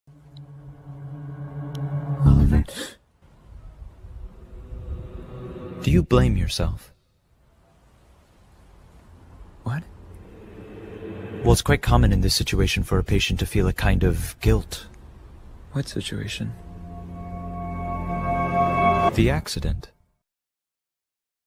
The Accident | OC PMV | Blood, Death, and Slight flash warning
AcrylicAnimations helped me this this!

Original audio: (Can't find it)

Programs used: Ibis paint X and Flipaclip